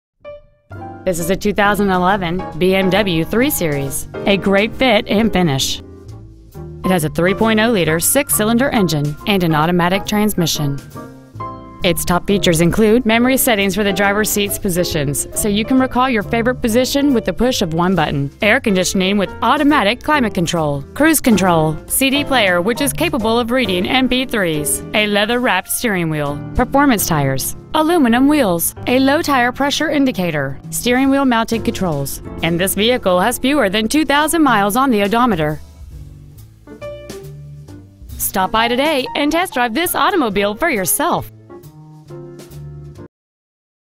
2011 BMW IN
We are proud to present this 2011 BMW .

 Year : 2011
 Make : BMW
 Model :
 Engine : 3.0L I6
 Trans . : Automatic
 Exterior : Blue
 Miles : 1,410
 Interior : Black
 BMW of Schererville
 1400 U.S. 41
  , IN 46375